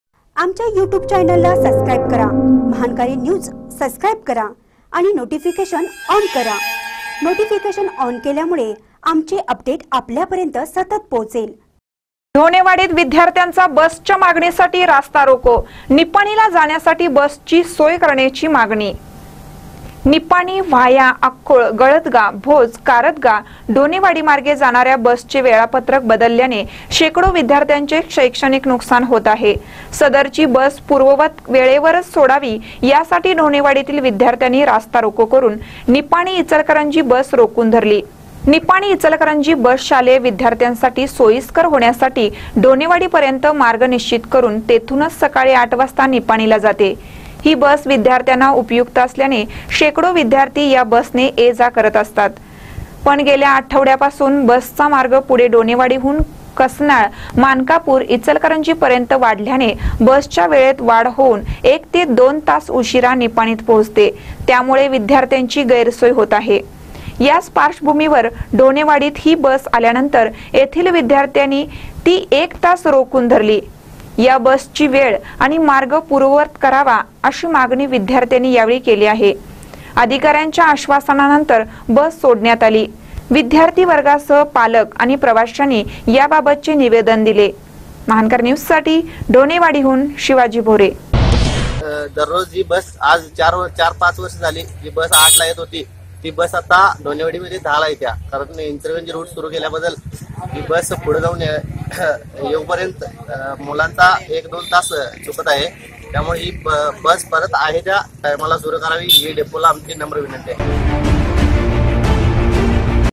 ढोणेवाडीत विद्यार्थ्याचा बसच्या मागणीसाठी रास्ता रोको
ढोणेवाडीत विद्यार्थ्याचा बसच्या मागणीसाठी रास्ता रोको : निपाणीला  जाण्यासाठी बसची सोय करण्याची मागणी